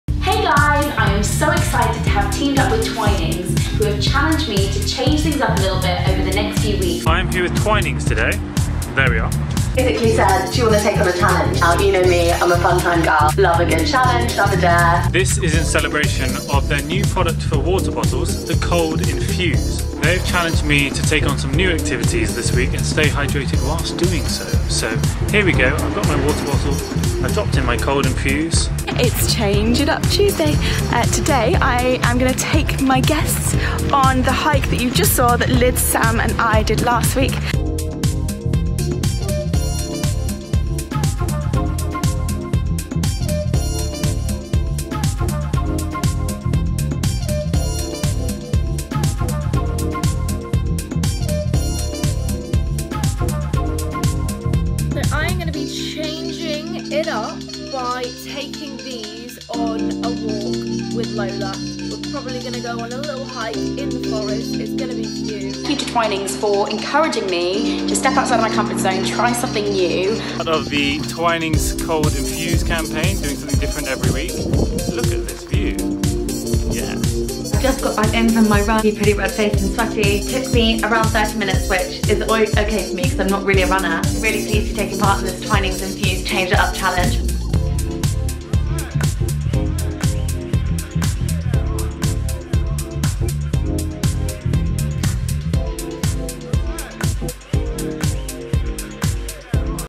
Twinings - Change It Up Challenge
Thanks to the ChangeItUp Challenge influencers who took part over the past month! Here are some of their best bits...

Pick up your Cold In'fuse here!